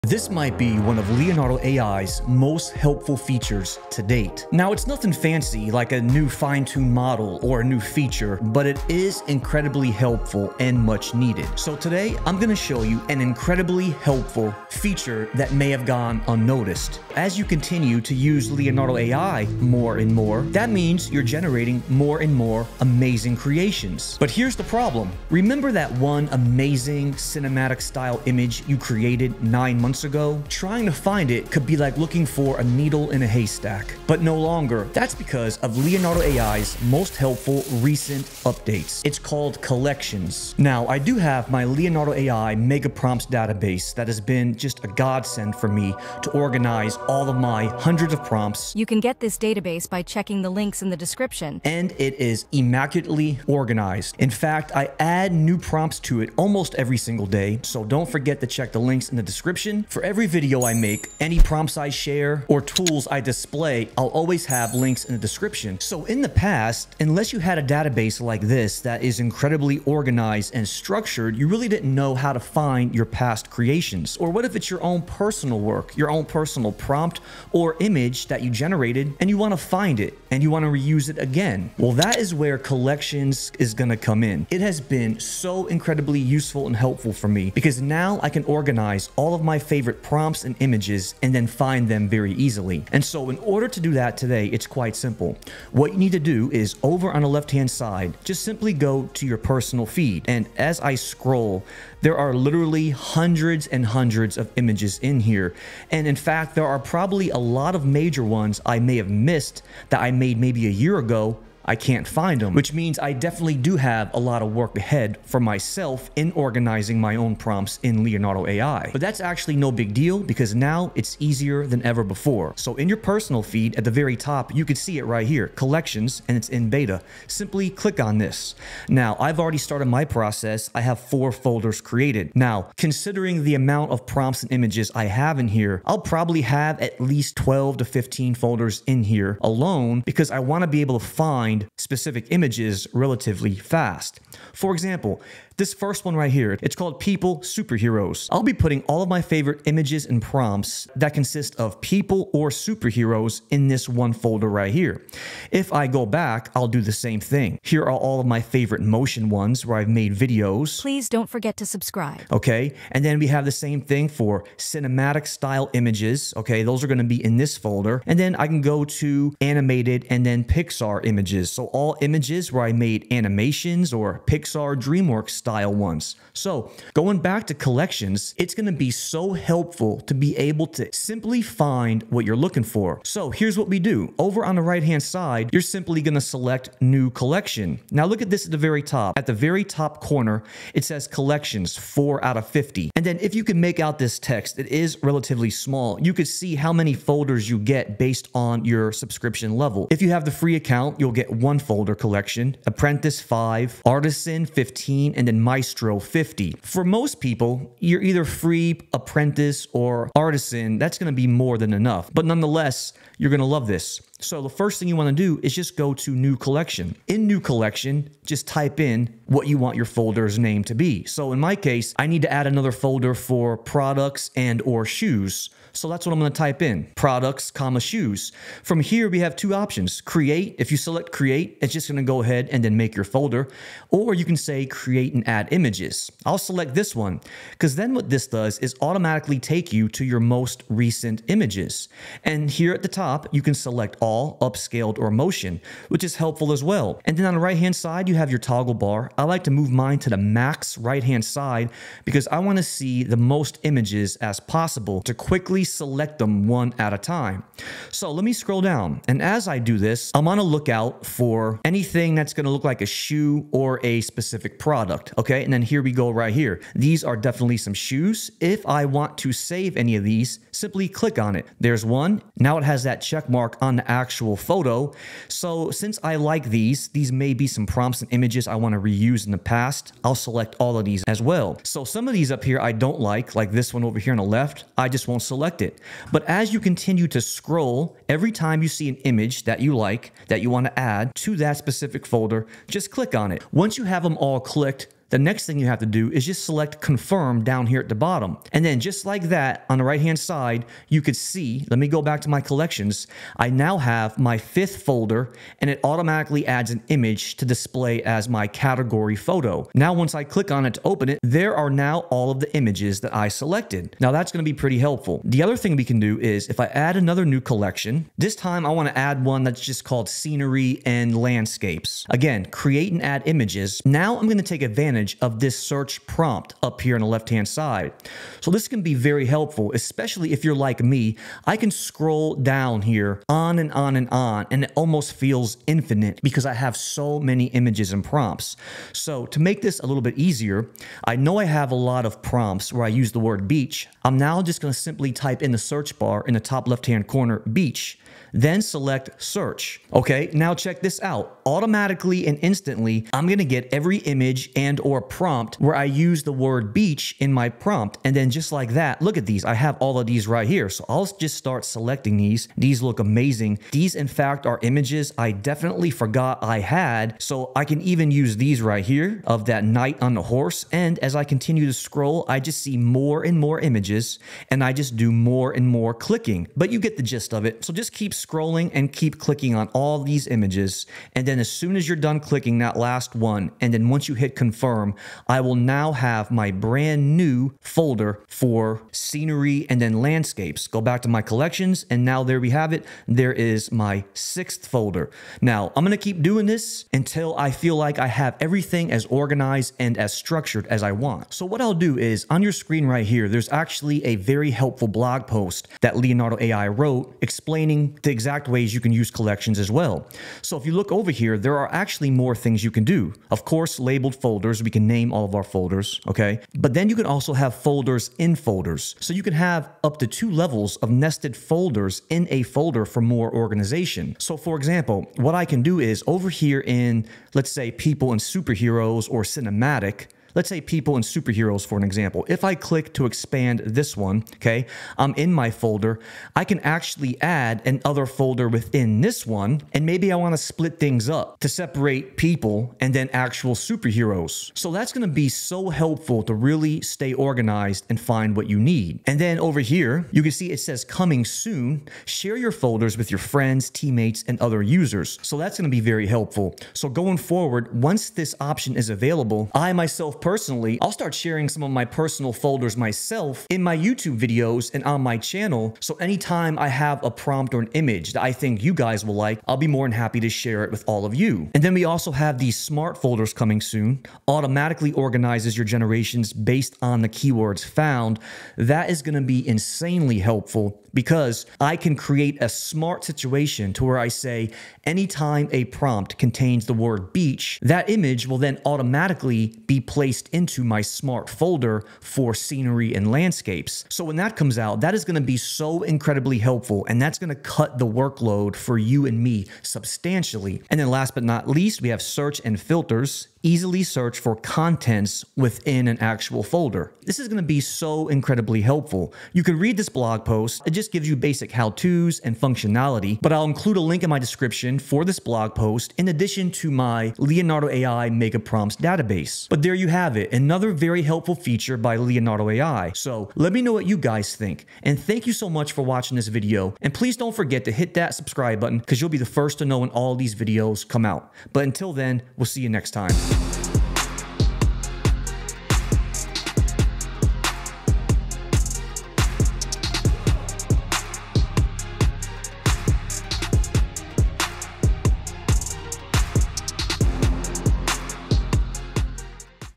The NEW, Helpful Leonardo AI Update You May Have Missed
Leonardo AI continues to release update-after-update with new features and tools that are very useful.

In this video, I'll show you one of the most helpful Leonardo AI updates that you may have missed. And it's actually an important one, especially when you continue generating multitudes of new images.

It's called Collections. Now, you'll be able to organize and find all of your prompts and images in the easiest and quickest way.

If you find my videos helpful, please consider hitting the THANKS button in any video or buy me a cup of ☕️ with a small tip